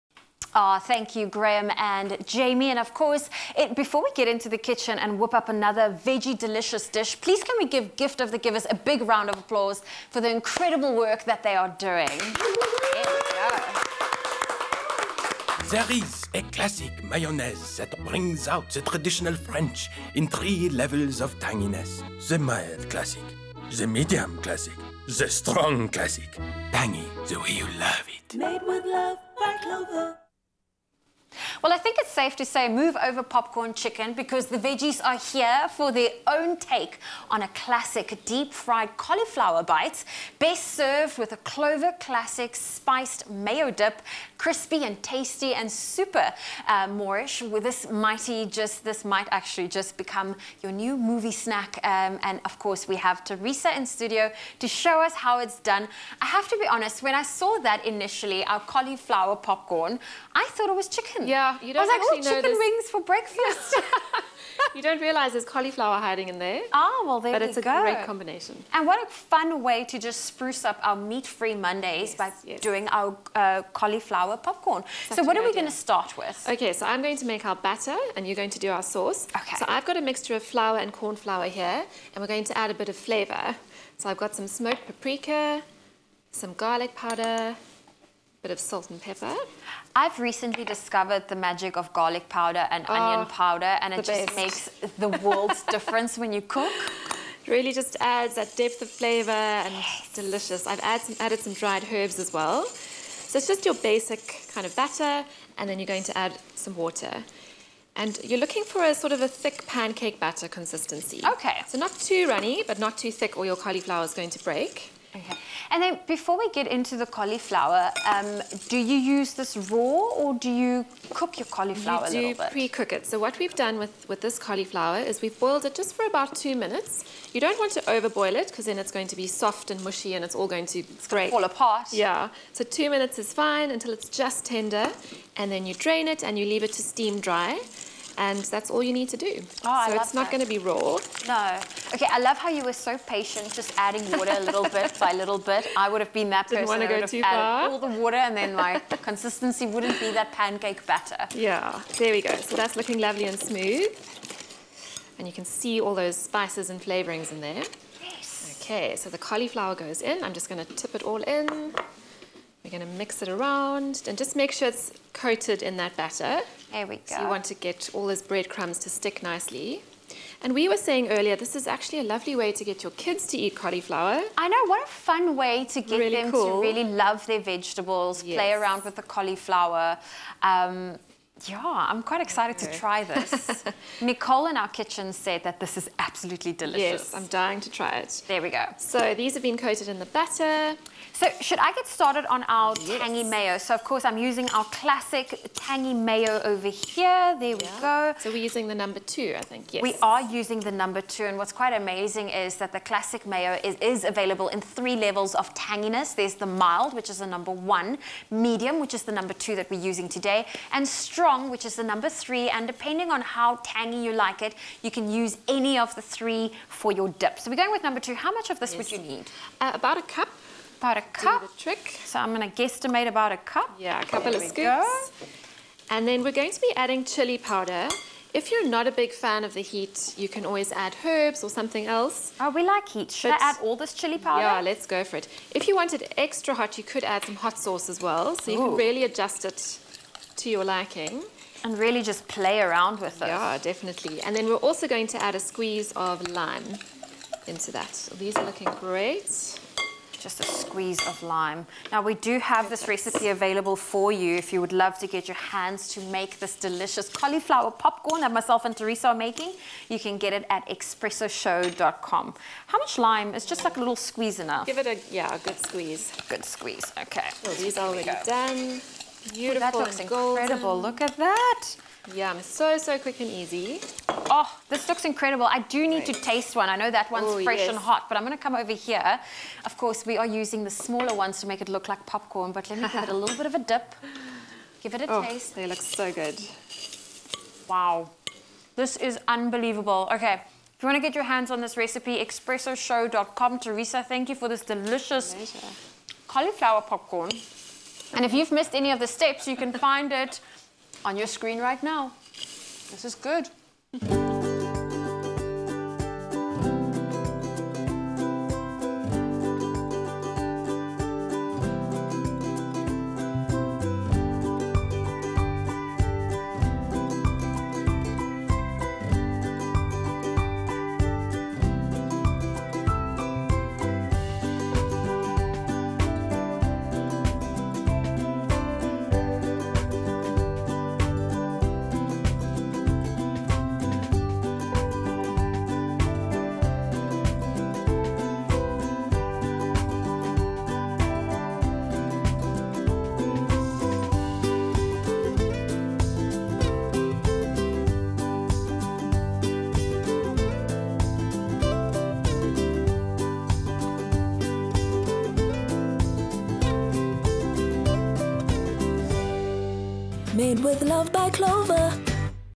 Recipe: Cauliflower popcorn with spiced Classic Mayo dip (CLOVER)
Try a veggie take on the infamous popcorn chicken, with deep fried cauliflower – best served with a Clover Classic spiced mayo dip. Crispy, tasty and super moreish, this might just become your new movie night snack.